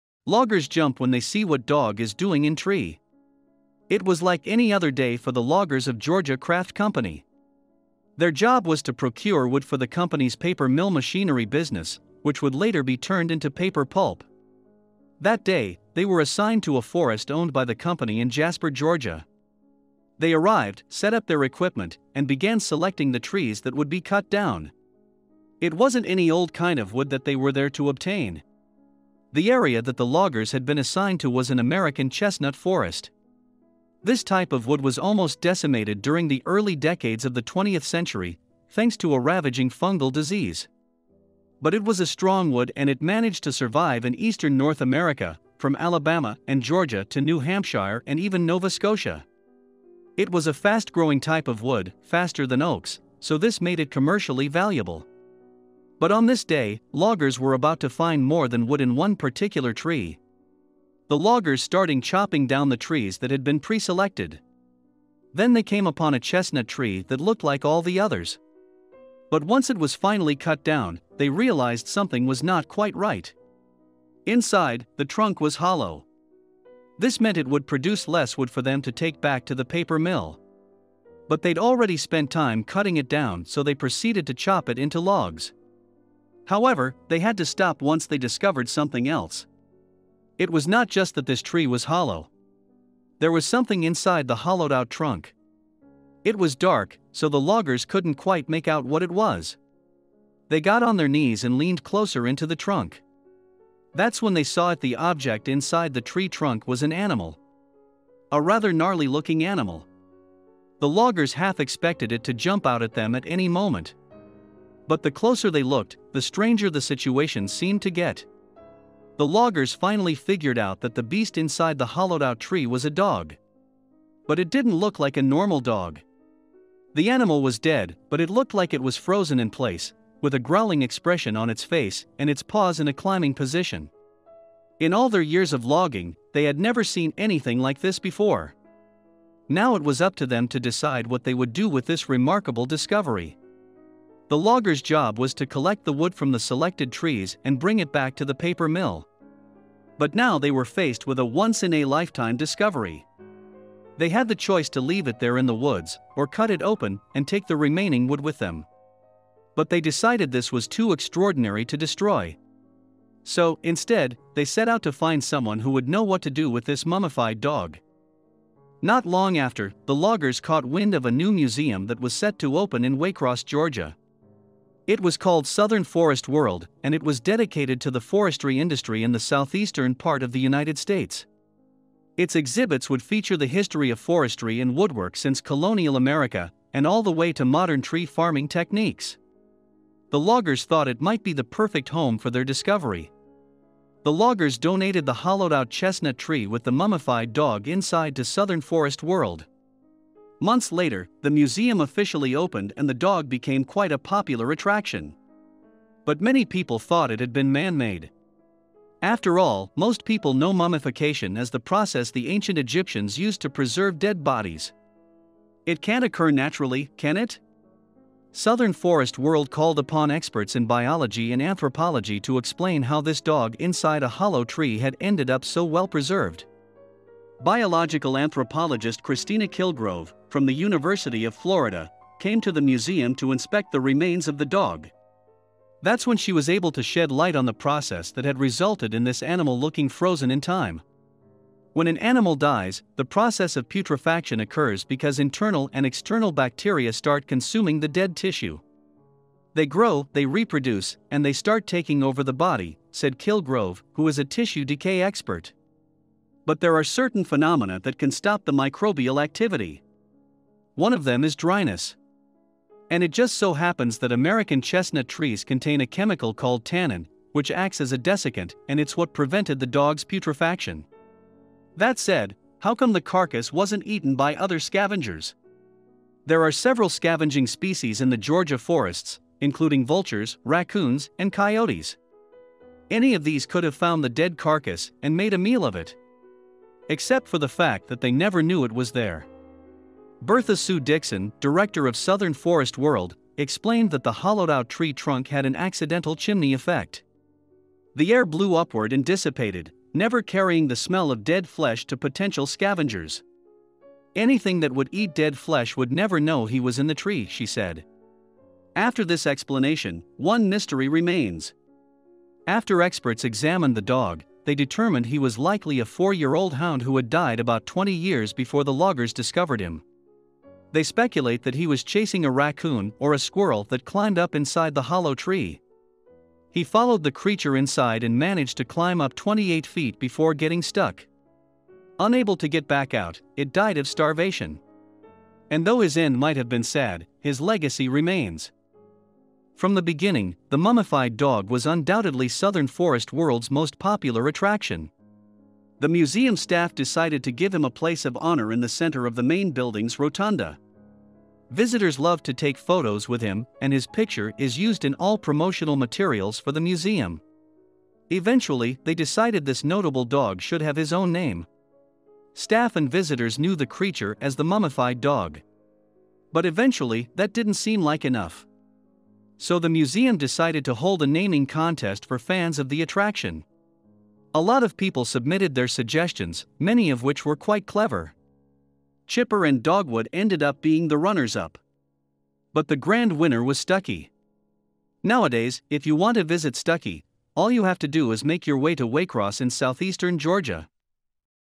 Loggers Jump When They See What Dog Is Doing In Tree
They jumped back, startled. For a second, they thought the creature was going to lunge at them. But when it didn’t, they leaned in to take a closer look. It was something like they had never seen before, and they couldn’t explain how it had gotten where it was.

They still had a job to do, but they desperately wanted to find out the truth about this discovery. And when they did, it turned out to be crazier than any of them imagined.